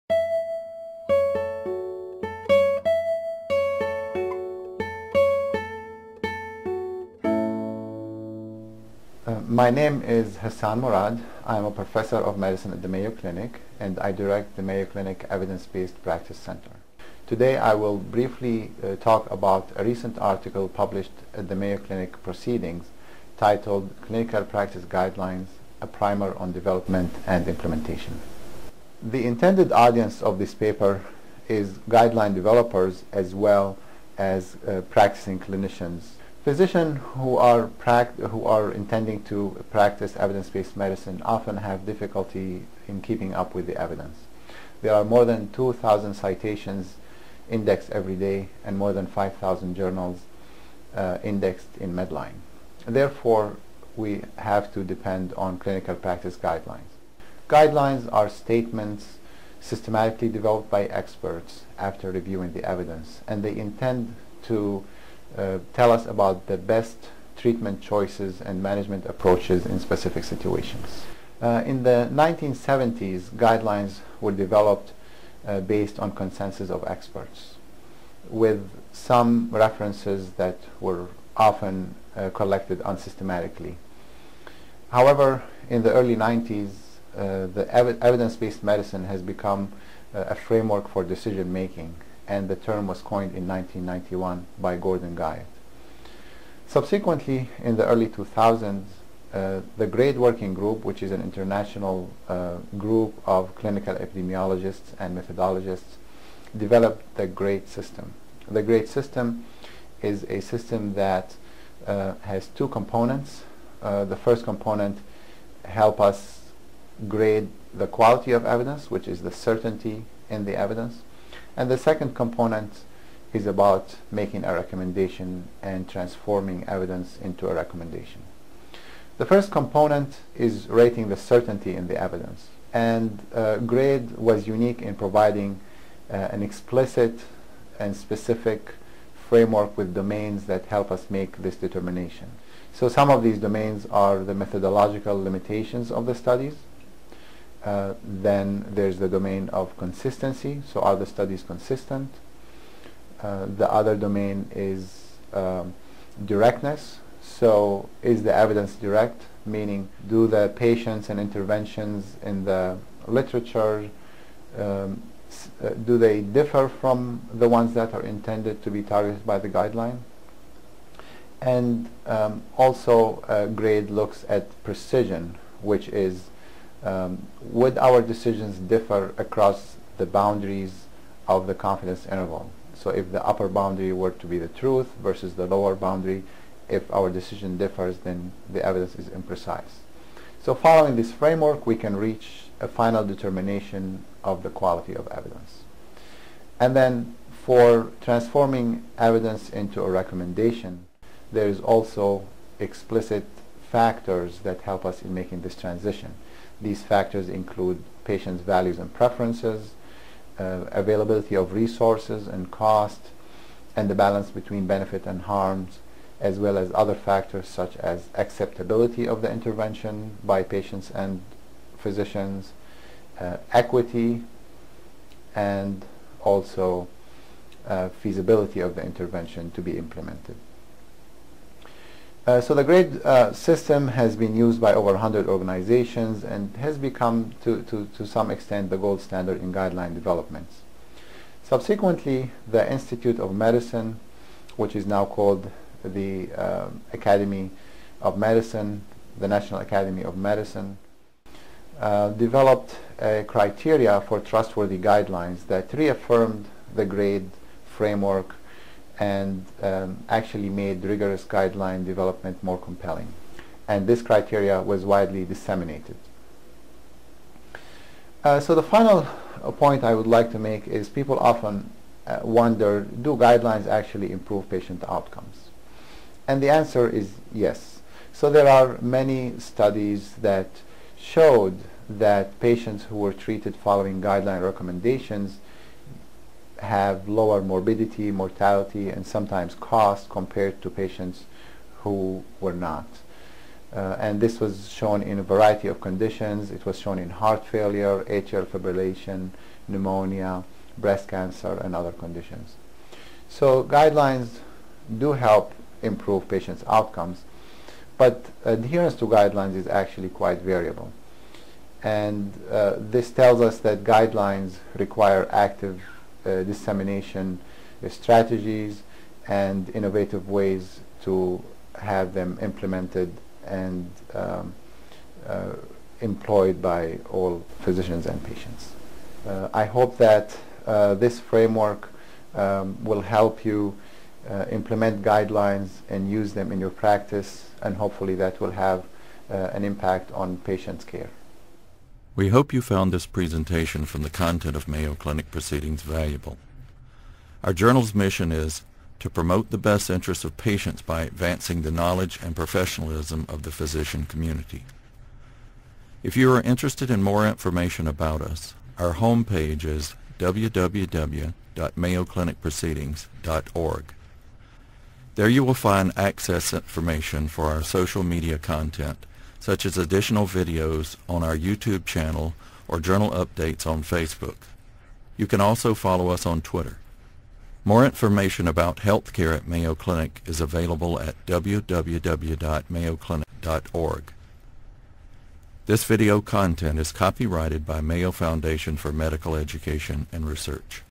Primer on Clinical Practice Guidelines
Dr. M. Hassan Murad, a Professor of Medicine and Director of the Evidence-Based Practice Center at Mayo Clinic in Rochester, MN, discusses his article appearing in the March 2017 issue of Mayo Clinic Proceedings, where he assists researchers and clinicians in understanding the concepts of quality of evidence and discusses how guidelines are developed and implemented with the ultimate goal of improving patient care.